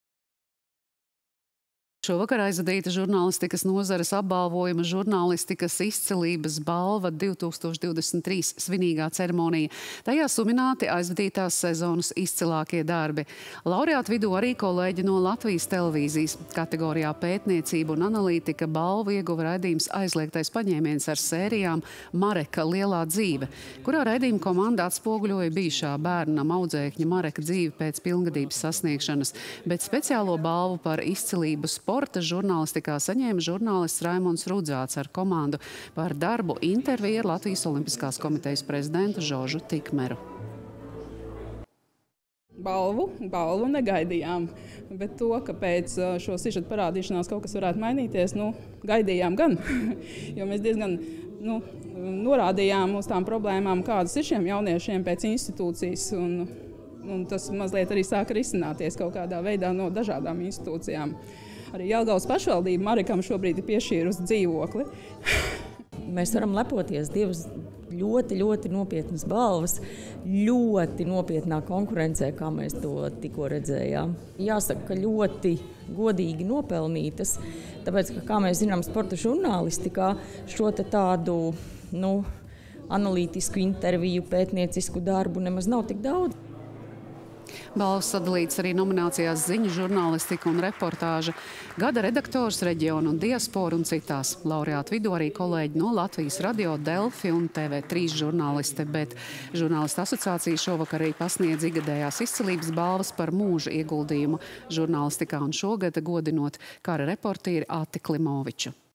Izcilības balvas žurnālistikā – arī LTV kolēģiem
Šovakar aizvadīta žurnālistikas nozares apbalvojuma “Žurnālistikas izcilības balva 2023” svinīgā ceremonija. Tajā sumināti aizvadītās sezonas izcilākie darbi.